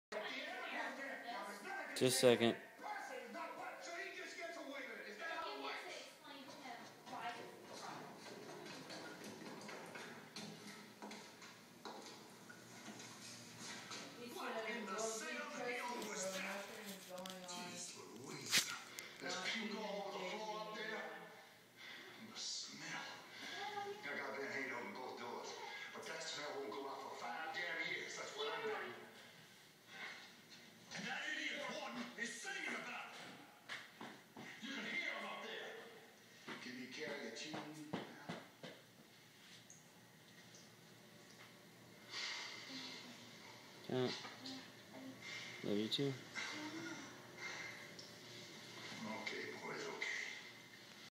Sleeping dogs lie
New puppy Copper runs in his sleep.